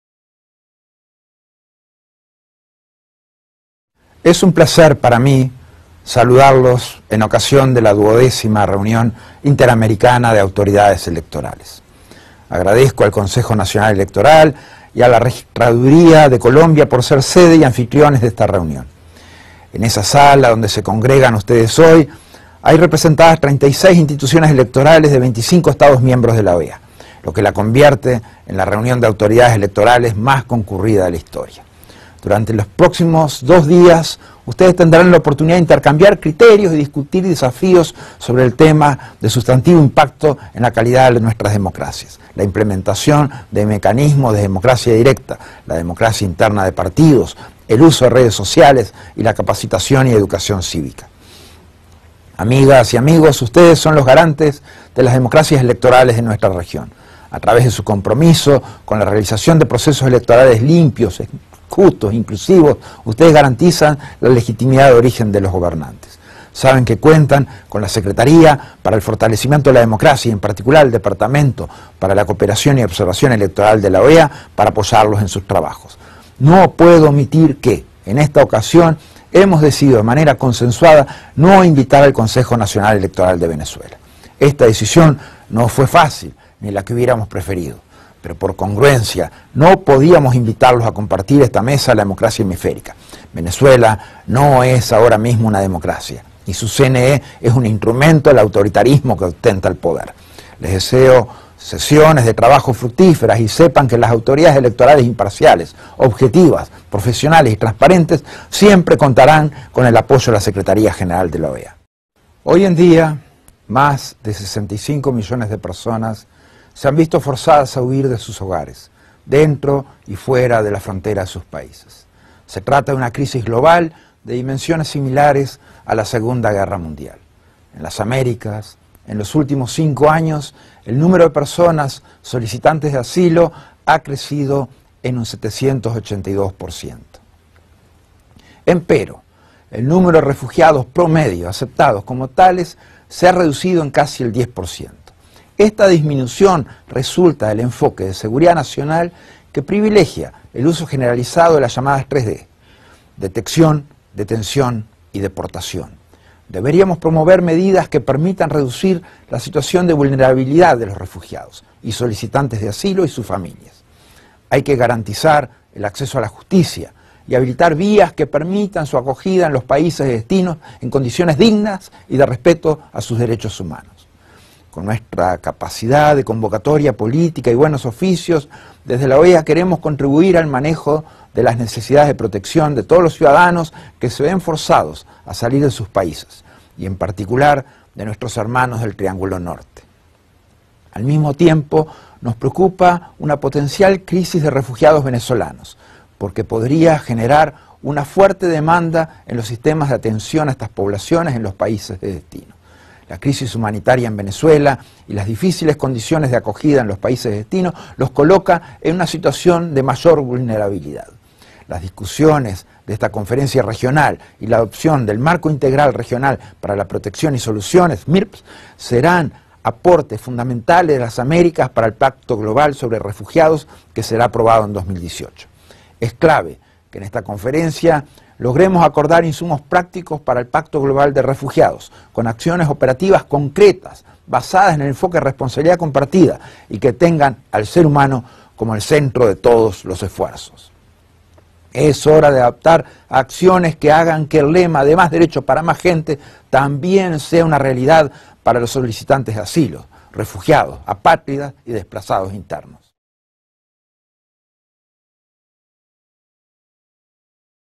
(26/10/2017) - LUÍS ALMAGRO: “Venezuela NO es una democracia” – CRISIS HUMANITARIA- (VG)
XII° Reunión de Autoridades Electorales OEA - Colombia -  Vía: OEA – (26 / Octubre / 2017) – VENEZUELA